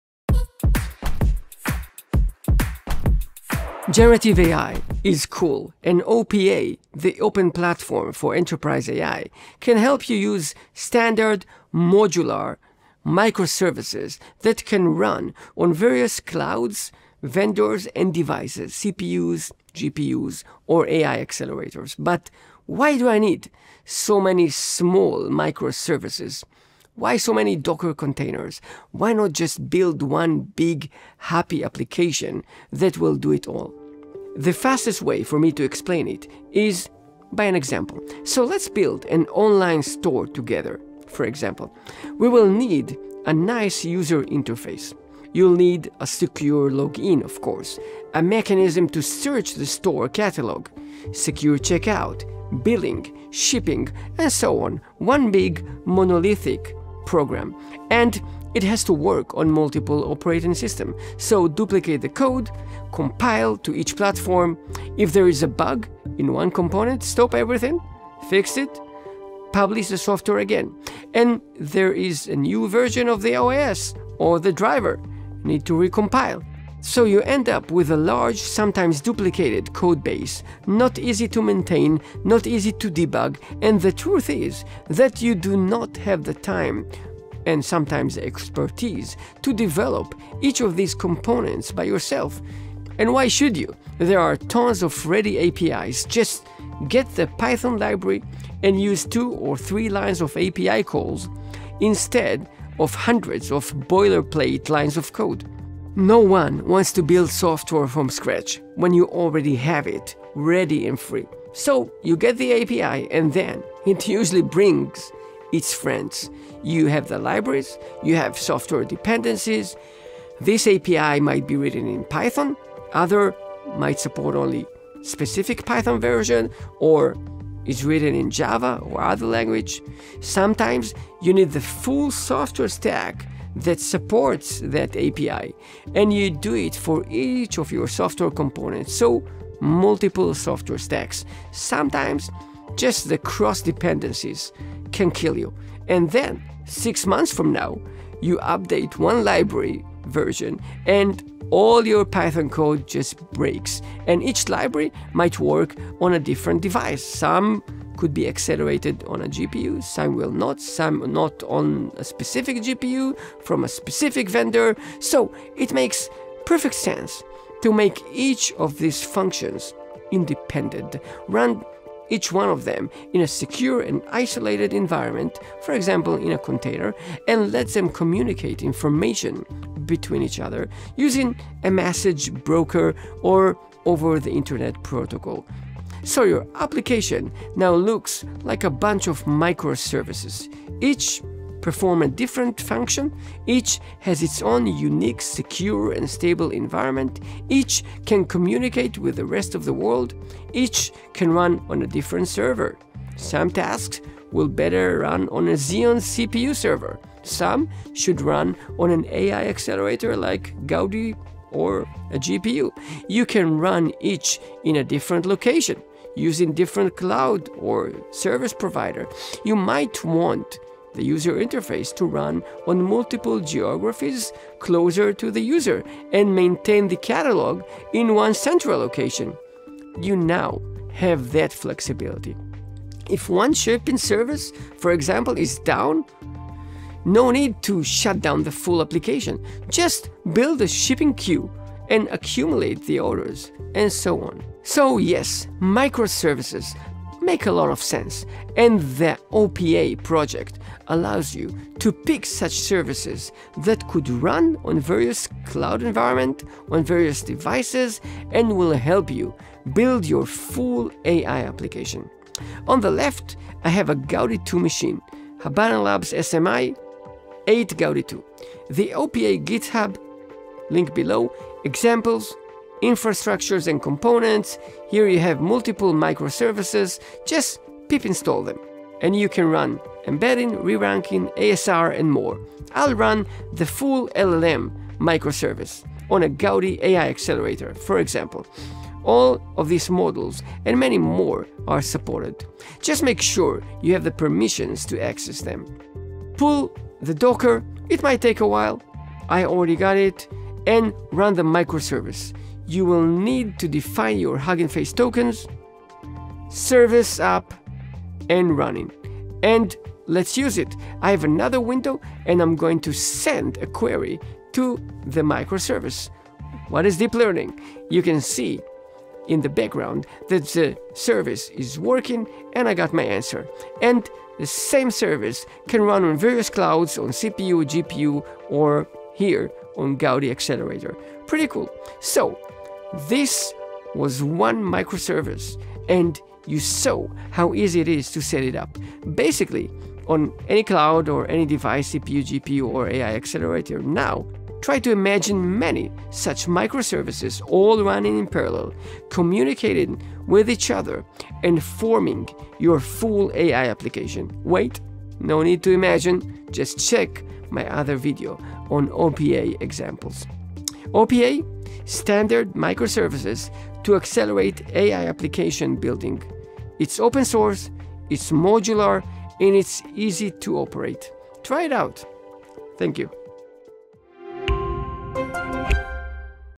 OPEA (Open Platform for Enterprise AI) micro-services | AI with Guy | Intel Software
Why is OPEA (Open Platform for Enterprise AI)  built out of micro-services (And not a full application)